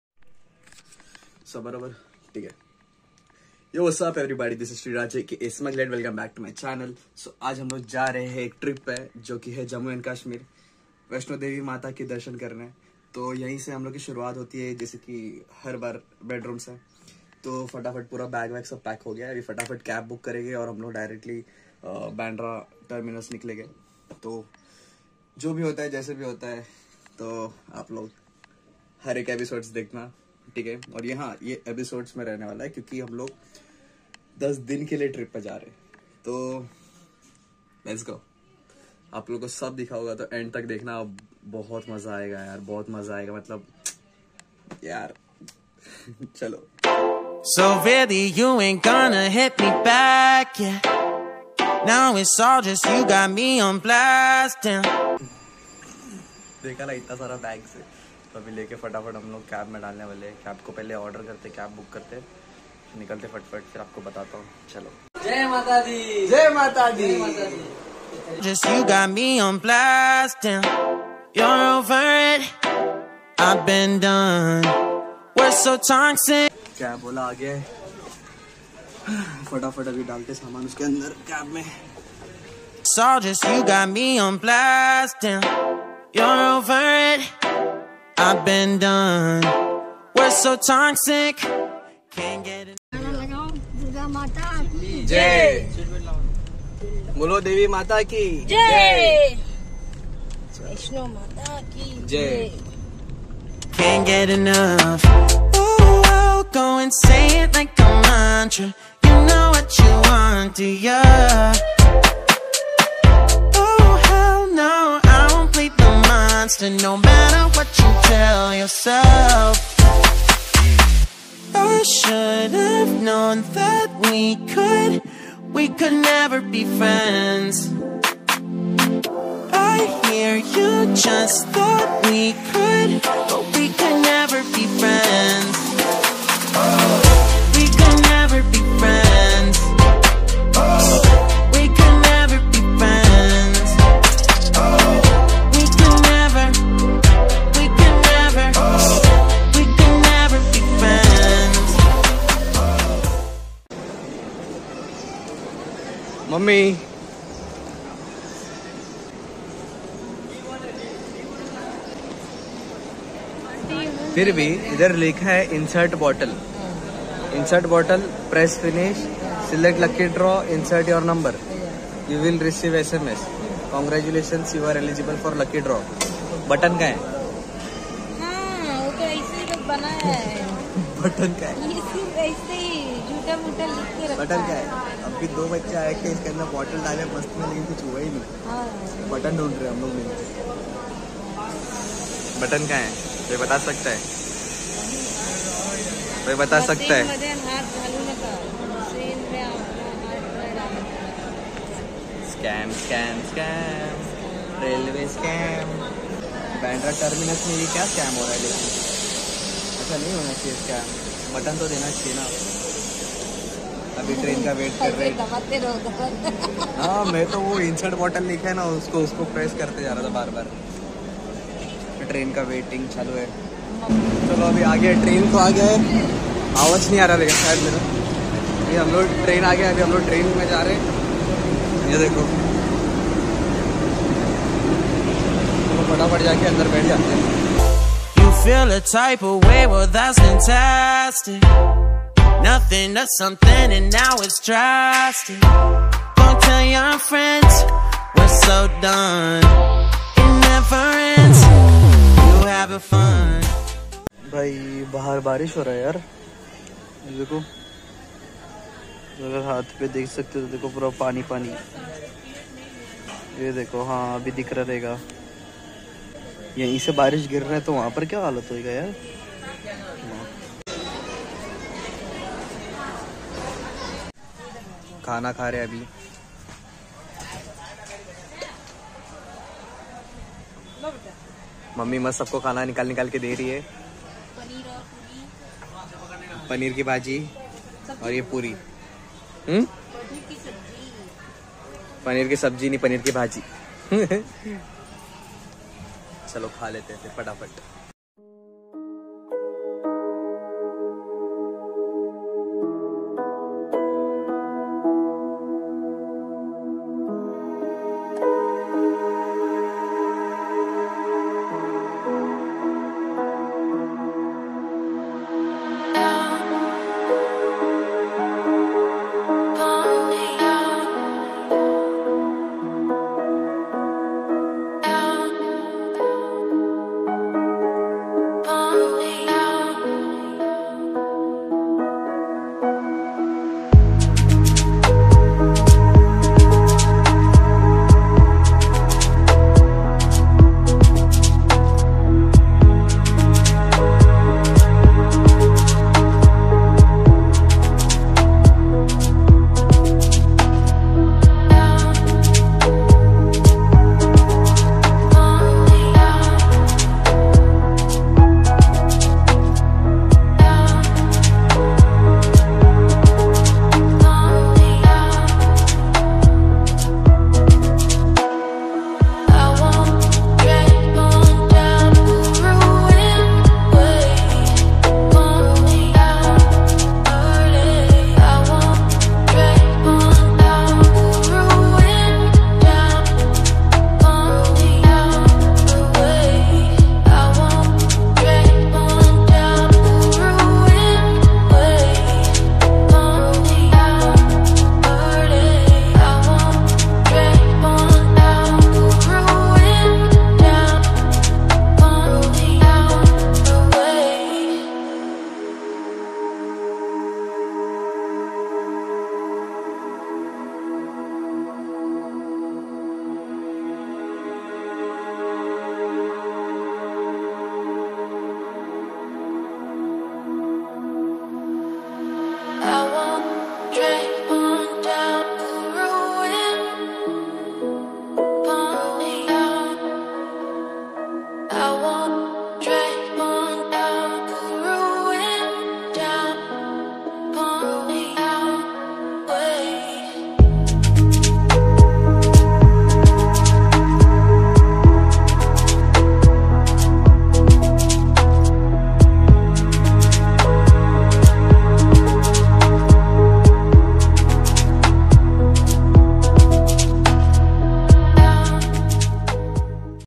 SHRI MATA VAISHNODEVI YATRA KE LIYE NIKAL HE GAYE🙏🏼😍| MUMBAI TO KATRA VAISHNODEVI MATA | 2023 | EP-1
SHRI MATA VAISHNODEVI YATRA KE LIYE NIKAL HE GAYE🙏🏼😍| MUMBAI TO KATRA VAISHNODEVI MATA|| SUMMER 2023

or @shriraj_a.k.a_smugglet

or @smugglet_art